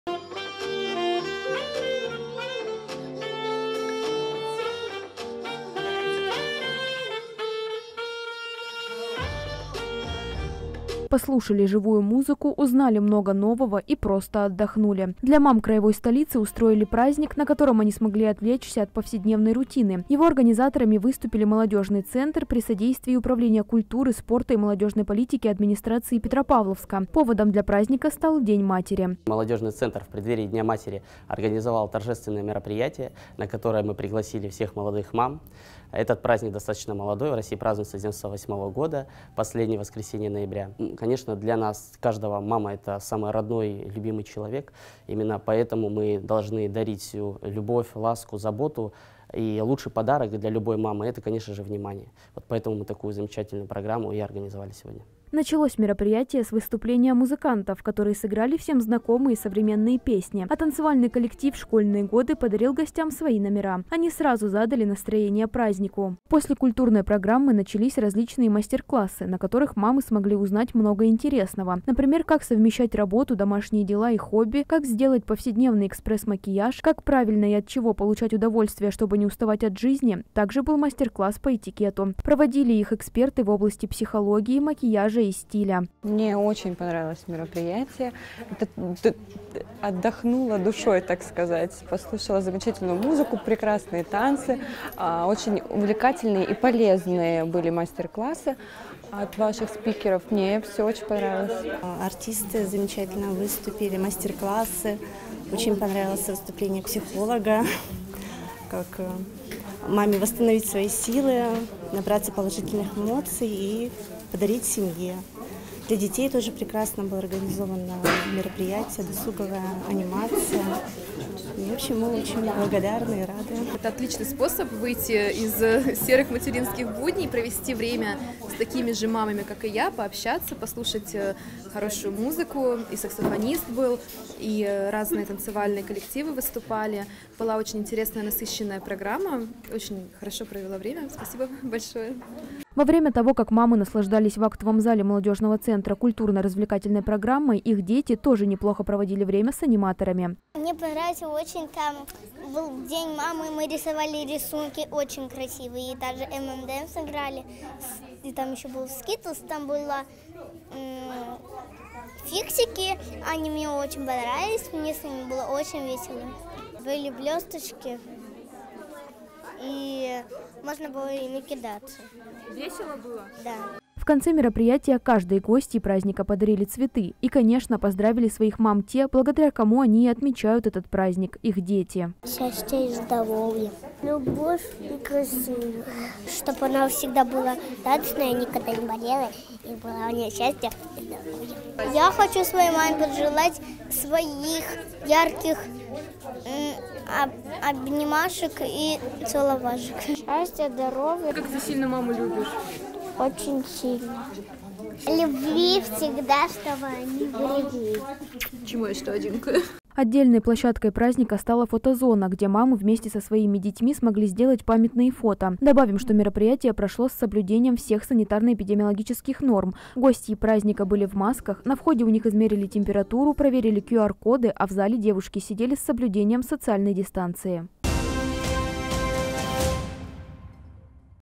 Праздник для мам | Новости Камчатки | Происшествия | Масс Медиа
Послушали живую музыку, узнали много нового и просто отдохнули – для мам краевой столицы устроили праздник, на котором они смогли отвлечься от повседневной рутины. Его организаторами выступили «Молодежный центр» при содействии Управления культуры, спорта и молодёжной политики администрации Петропавловска. Поводом для праздника стал День матери.